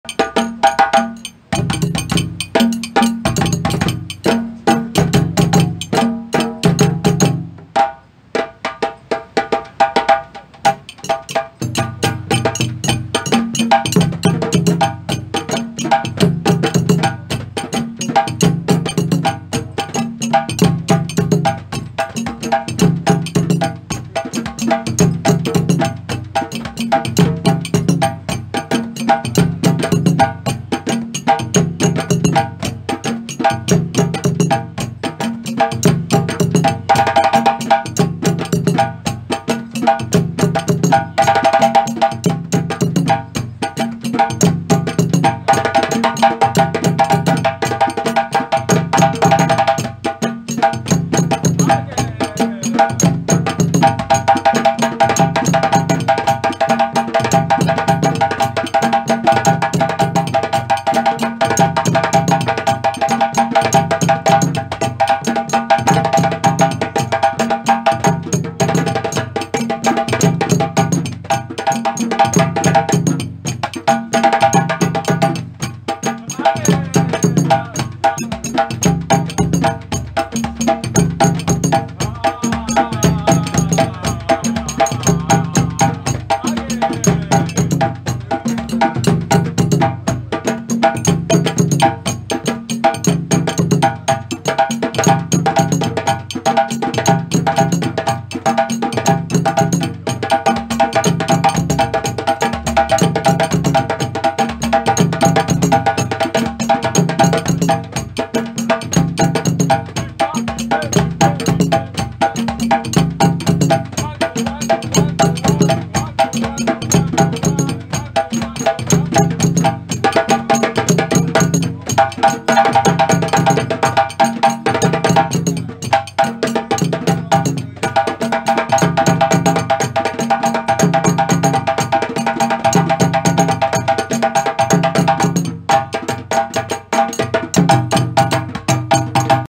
LAFE. Cours de rythmes mandings By BOGA SENE
Cours by Boga Sene with Almami and Amet
Yoff BCAO, Dakar 2019